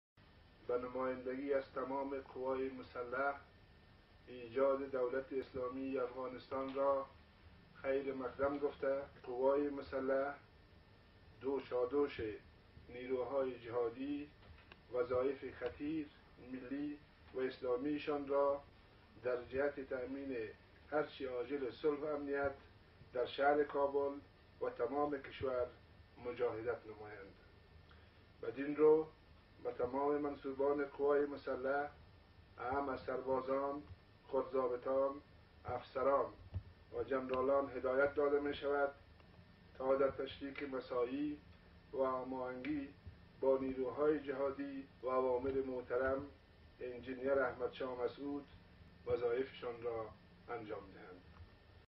Nabi Azimi - Ahmad Shah Masoud
فرمان تسلیمی قوای مسلح جمهوری افغانستان توسط سترجنرال محمد نبی عظیمی معاون وزیر دفاع به نمایندۀ آی ایس آی پاکستان در کابل - احمد شاه مسعود:
((به تمام منسوبان قوای مسلح اعم از سربازان، خرد ظابطان، افسران و جنرالان هدایت داده میشود تا در تشریک مساعی و هماهنگی با نیروهای جهادی و اوامر محترم انجنیر احمد شاه مسعود وظایف شان را انجام دهند.)) نبی عظیمی.
نوت: راستی، احمد شاه مسعود انجنیر بود؟ کدام انجنیر و از کدام پوهنځۍ انجنیری (پوهنتون کابل و استیتیوت پولیتخنیک) و در کدام سال فارغ شده بود؟؟